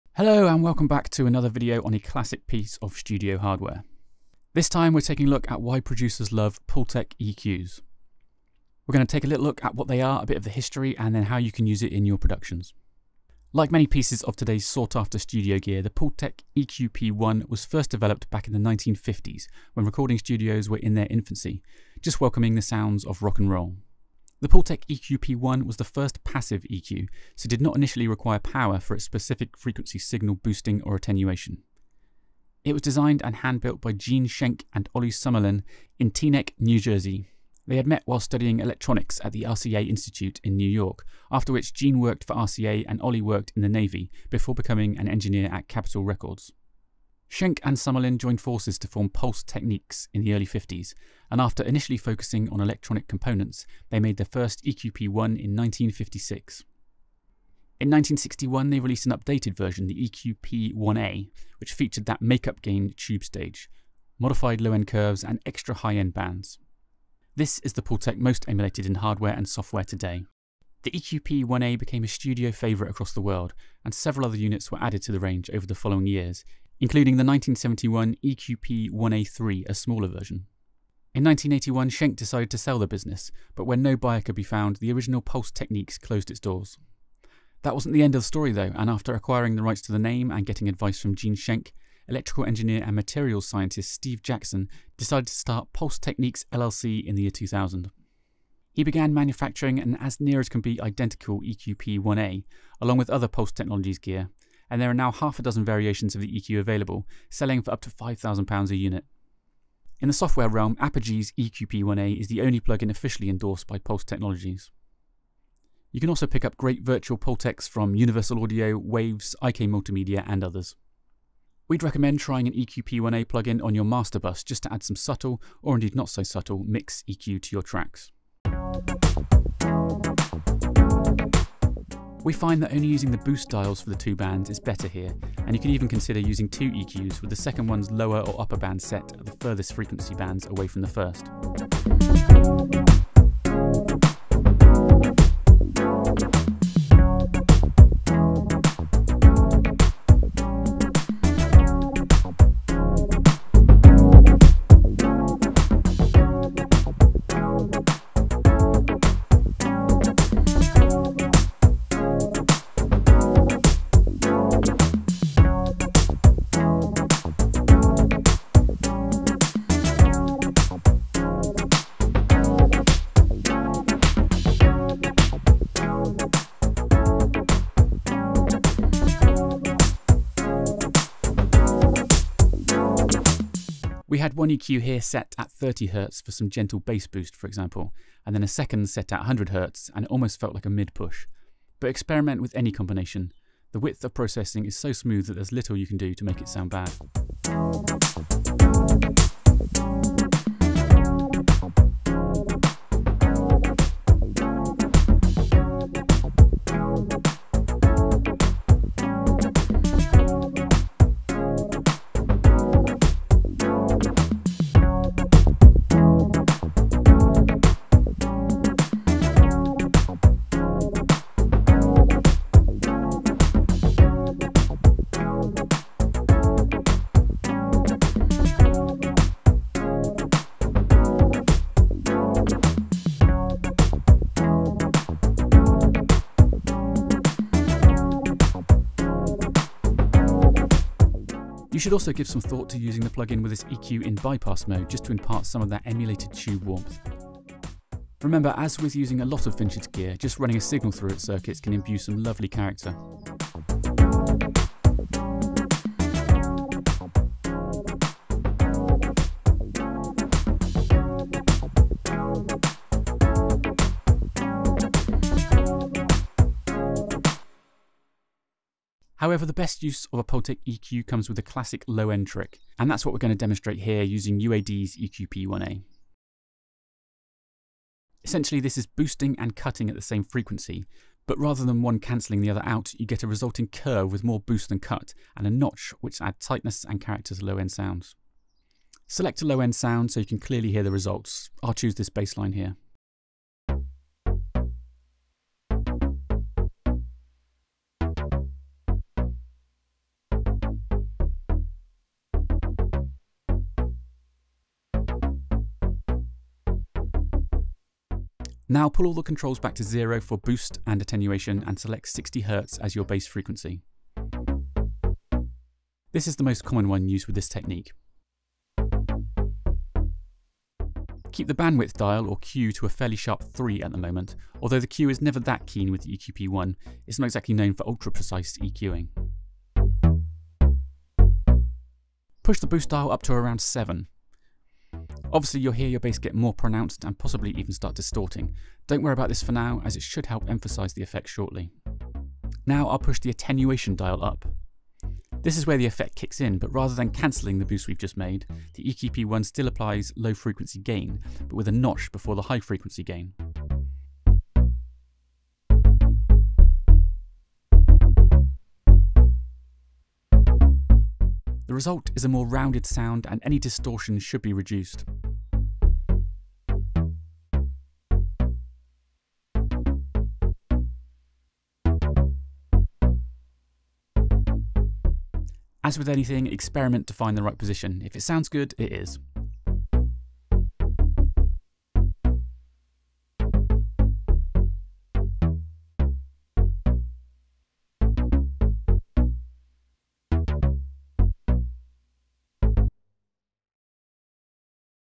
Why producers love the Pultec EQP 1A... and how to use it in your DAW
This 1950s EQ is still one of the most in-demand processors around, so much so that you can find many (many) hardware and software emulations. But just why is the Pultec EQP-1 such a revered machine? In this video we explain the history of the original and show you how the classic 'low-end trick' can help enhance kicks and bass, whether you're using hardware or a plugin recreation.